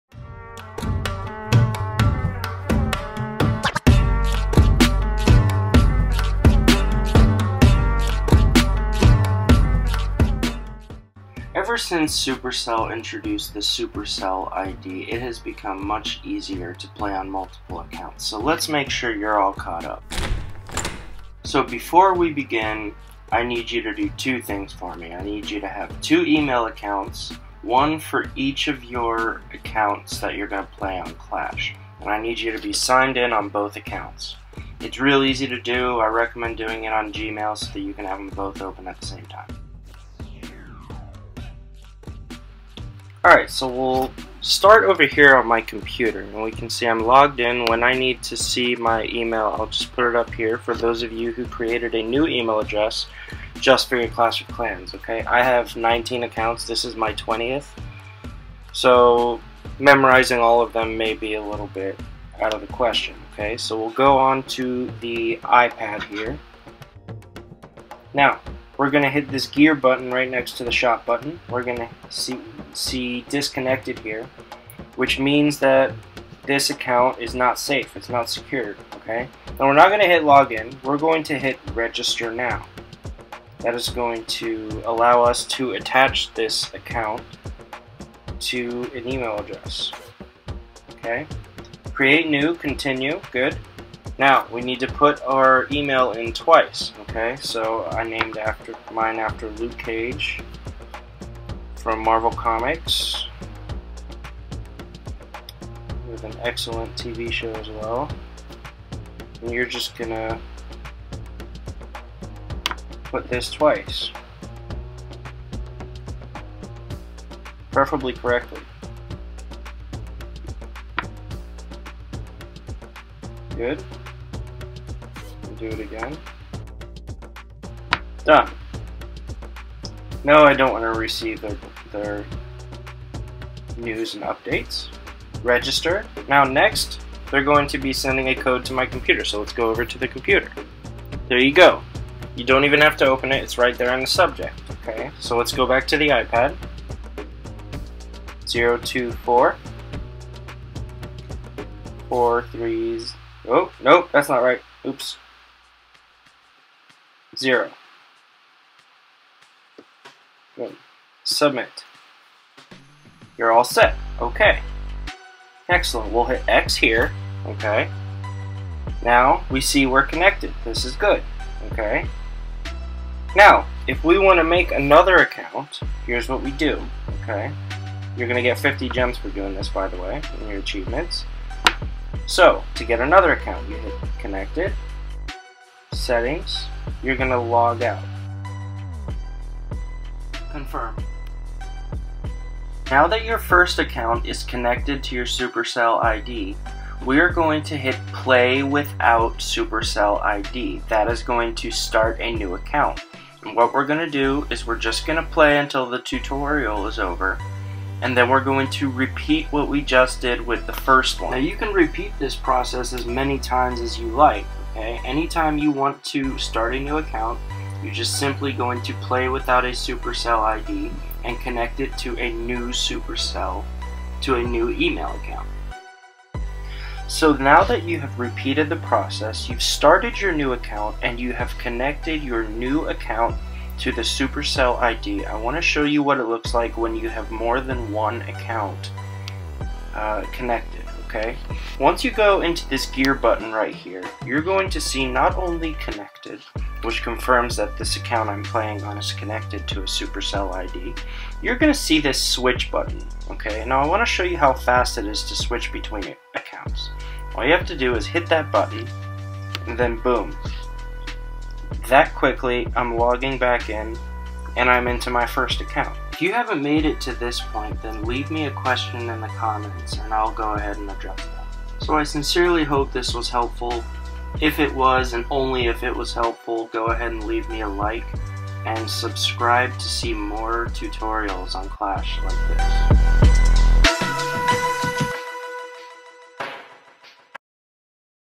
How to play on multiple accounts. Supercell ID made easy!
For those of you who haven't figured out how to play on multiple accounts or even anyone who hasn't linked their own account to a supercell ID. This step-by-step guide will make it easy. Before you hit play register 2 e-mail accounts and log into them (doesn't matter what kind of email). This should work equally well with either Apple or Droid devices.